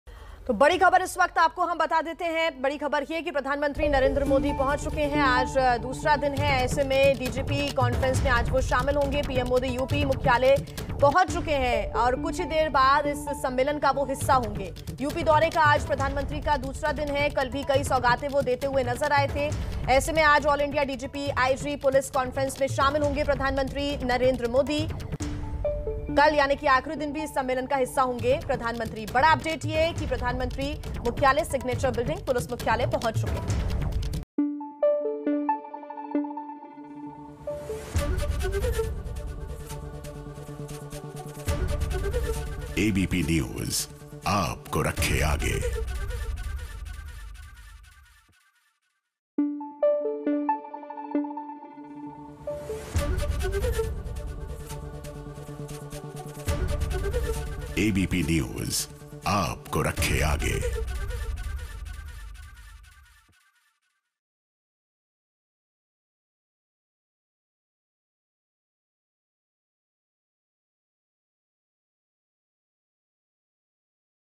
PM Modi UP Visit: यूपी पुलिस मुख्यालय पहुंचे पीएम मोदी
प्रधानमंत्री नरेन्द्र मोदी आज और कल यानि 21 नवंबर को पुलिस मुख्यालय लखनऊ में पुलिस महानिदेशकों (डीजीपी) और पुलिस महानिरीक्षकों (आईजीपी) के 56वें सम्मेलन में भाग लेंगे. शुक्रवार को केन्द्रीय गृहमंत्री अमित शाह ने सम्मेलन का उद्घाटन किया था. आज और रविवार को प्रधानमंत्री नरेन्द्र मोदी इस बैठक में शामिल होंगे.